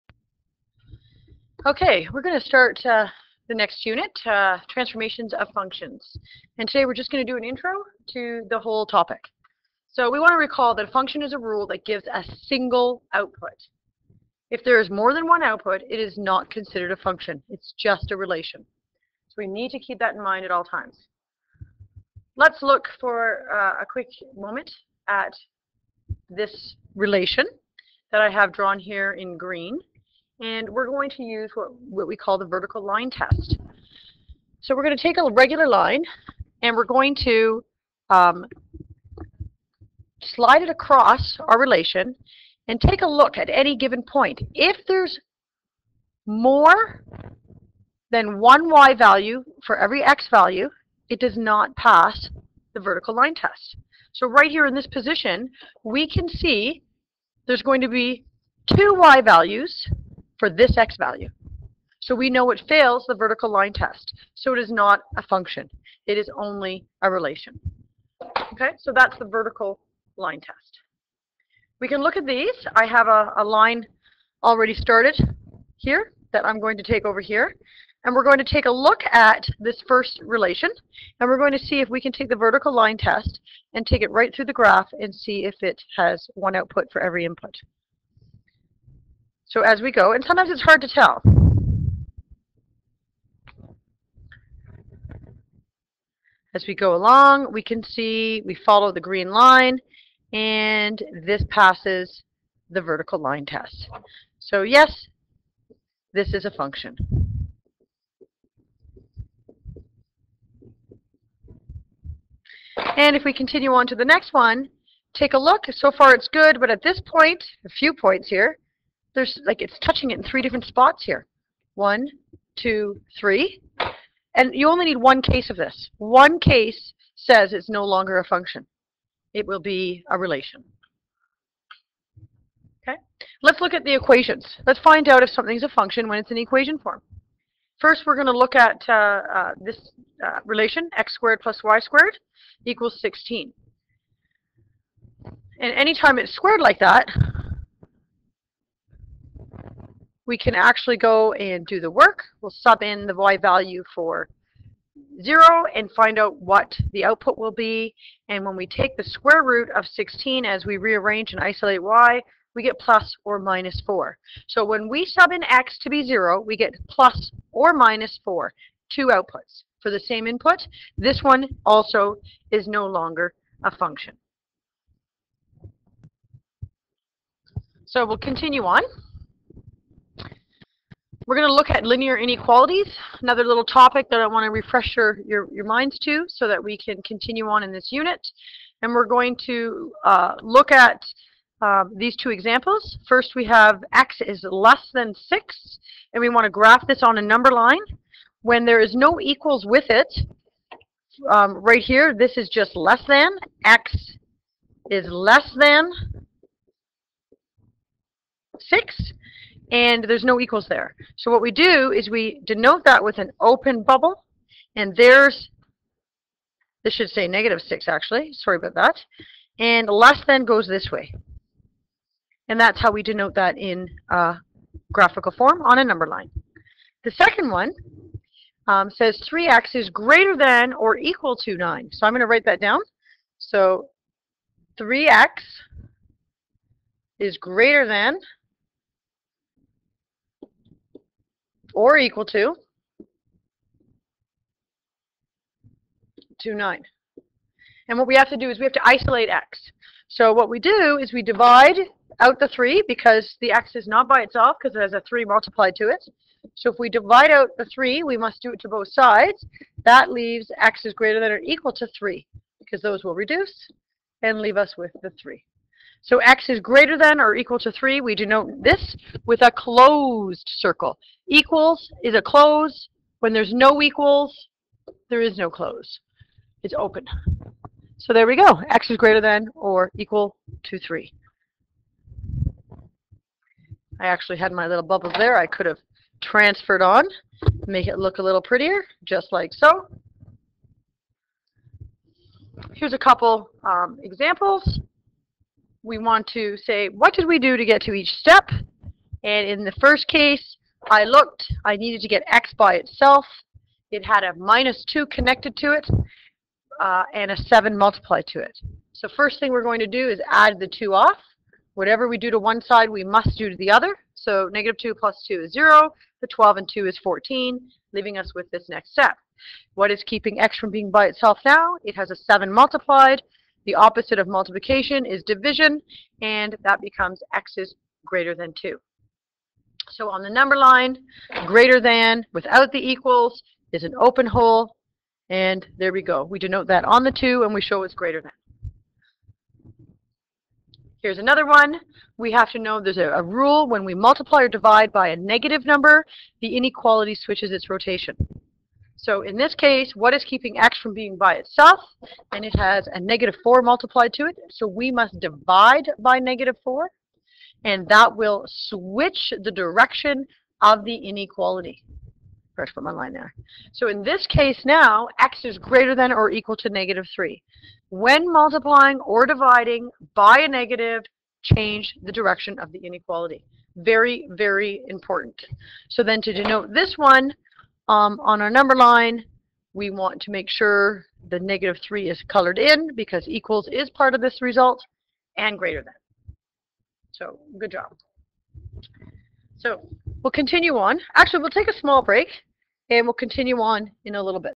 Intro To Transformation Lesson - On SmartBoard
Intro To Transformation Lesson - On SmartBoard. Recorded with SmartBoard recorder by Ms. Shank for Grade 11 University Math course.